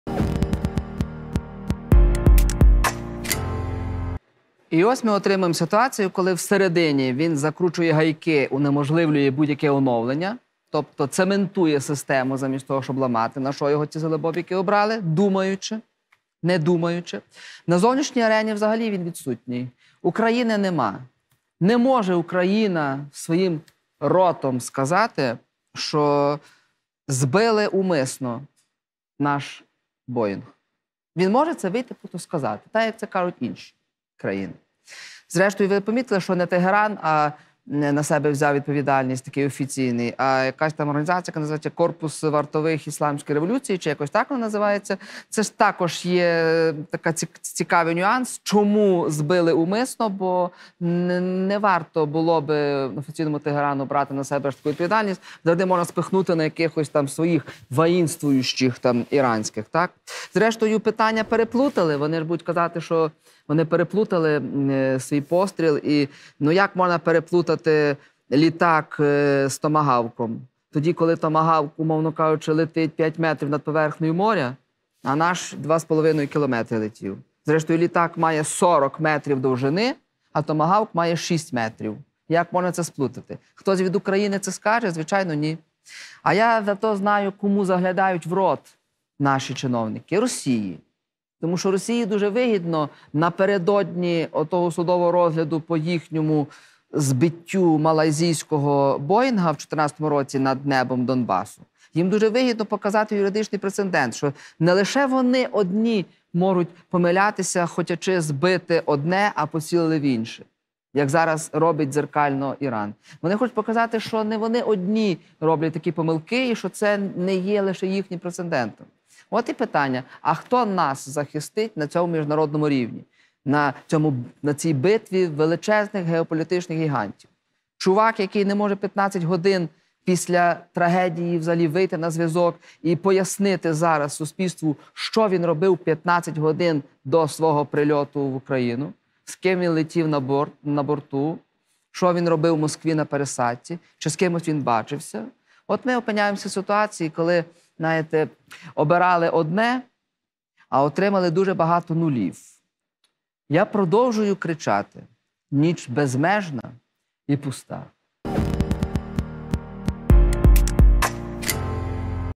💥 "Президенте Зеленський, а що ви робили у Москві?", - Остап Дроздов
💥 "Президенте Зеленський, а що ви робили у Москві? Чому не було реакції щодо трагедії в Ірані?", - Остап Дроздов